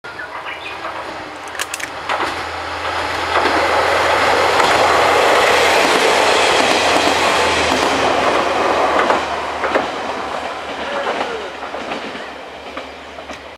懐かしの国鉄一般色風塗装キハ47ノスタルジーとキハ40国鉄一般色風塗装との交換風景
津山線牧山駅で列車交換する国鉄一般色風塗装時代のキハ47(現在は、国鉄急行色風塗装)と増結用に国鉄一般色風塗装に変更されたキハ40